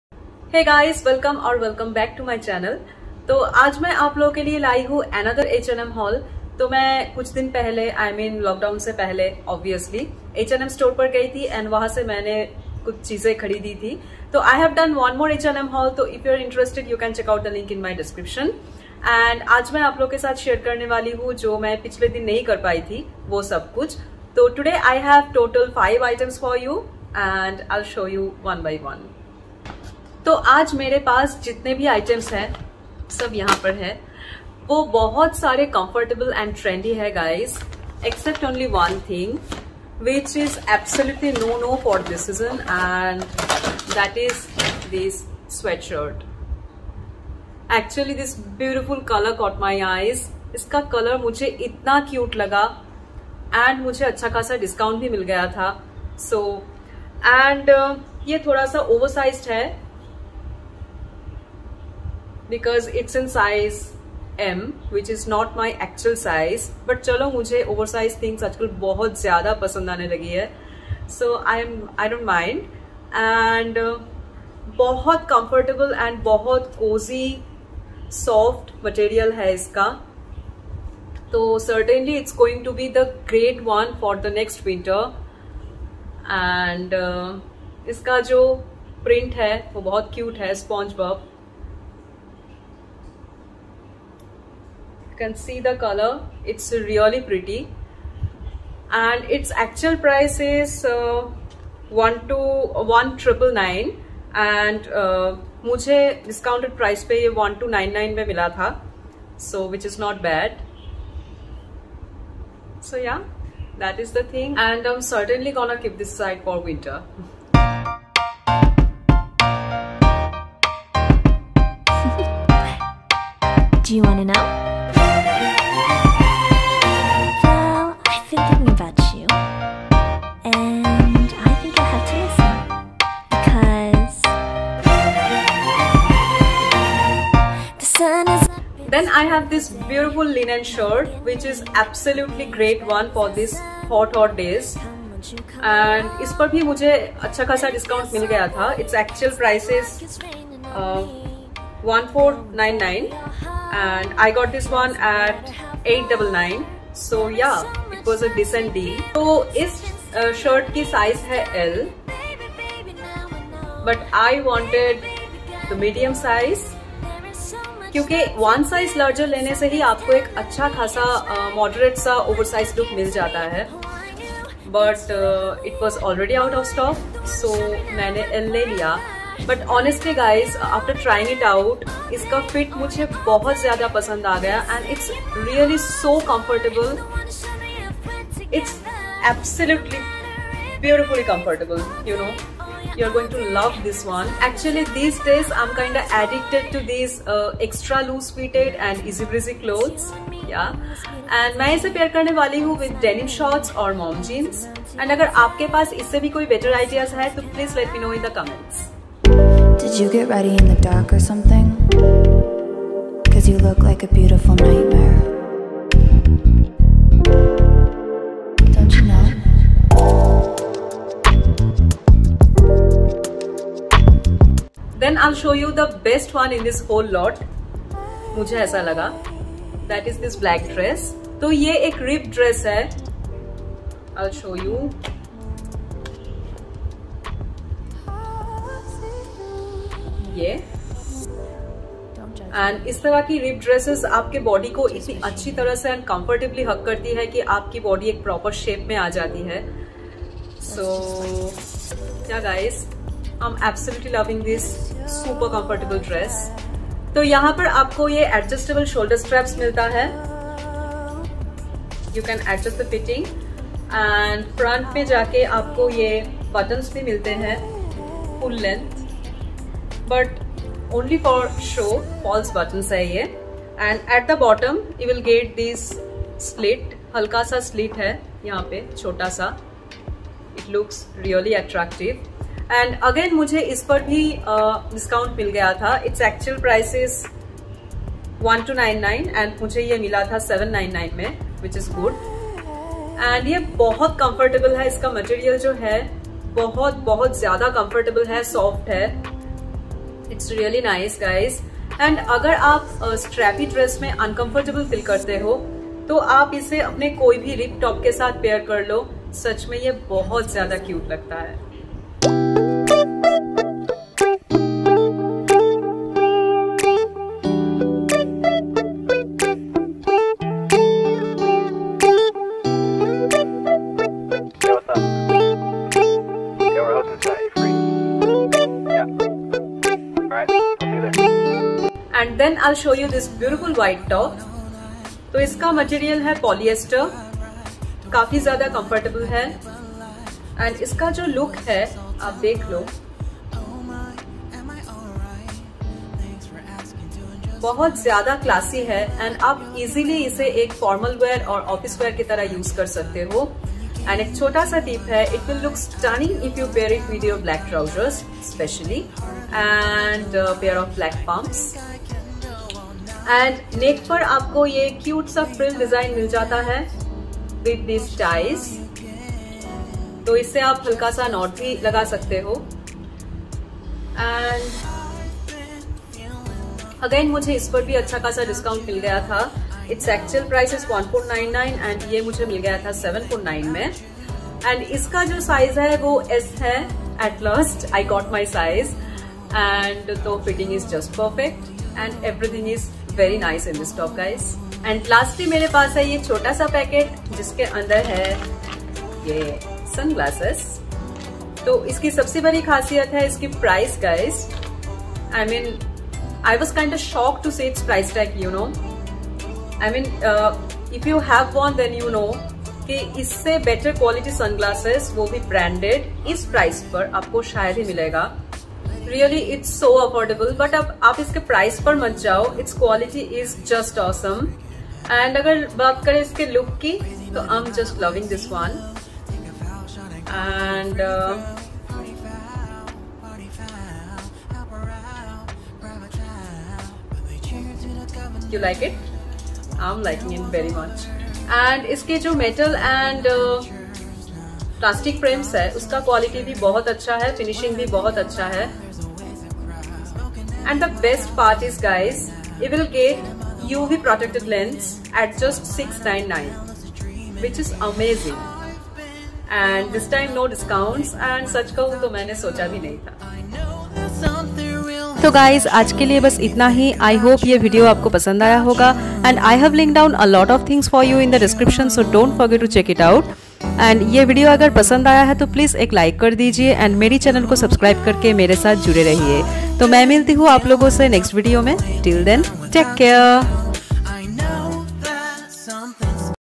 H&M affordable sale haul/Tops, Dress, Sweatshirt, Accessories/H&M Kolkata Store Haul/what tanumeans
Hi friends ☺️ today I'll be sharing some beautiful trendy outfits from H&M Kolkata Store. Hope you'll enjoy watching it 😊 I've linked down some similar items for you, if you are interested can check these out...